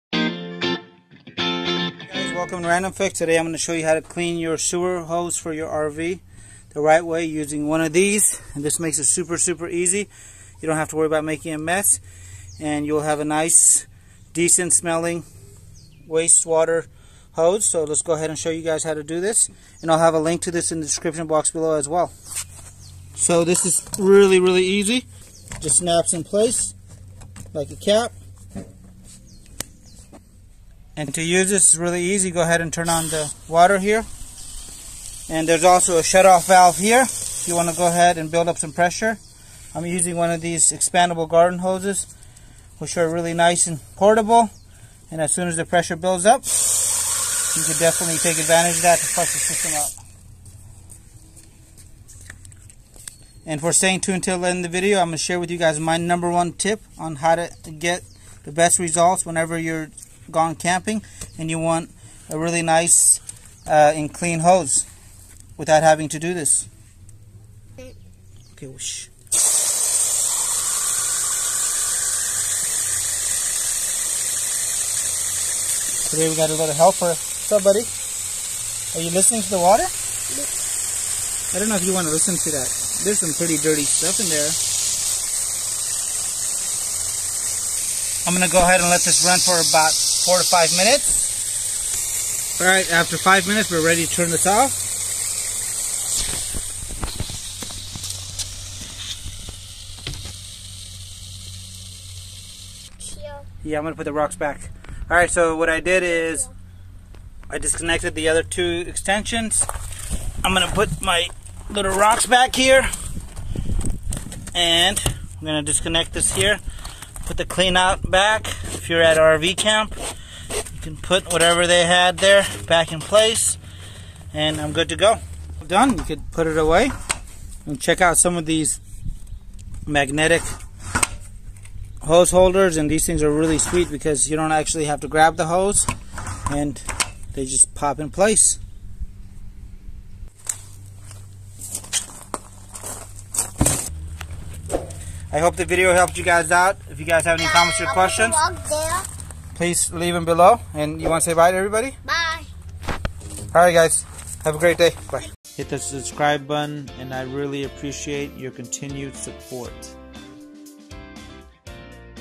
▶️How To Clean Your RV Sewage Hose HOW TO POWER WASH RV Disposal Line
▶️▶️Wondering how to clean your RV sewage hose after your weekend camping? In this episode, Randomfix will teach you how to clean your RV sewage hose in a few easy steps.

▶️recommended RV sewage gravity supports
▶️recommend rv bumper endcaps
▶️recommended RV sewage hose
▶️Recommended sewage wash cap